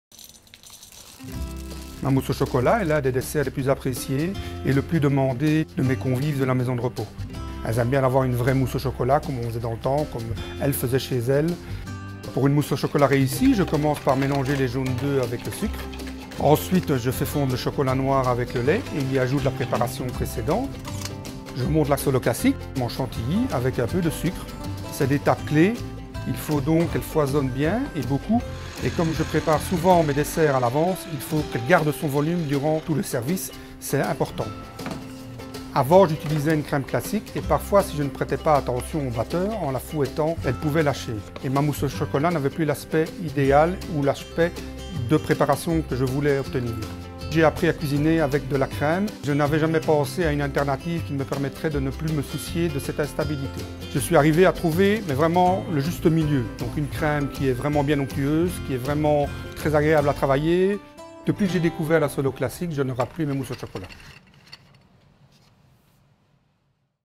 BENL - Solo Classique
Solo Classique:
- Tot 50% meer opgeklopt volume dan een klassieke vloeibare room
- Stabiel in zure bereiding
- Stabiel in bereidingen met alcohol
- Stabiel bij invriezen
- Zowel koud als warm te gebruiken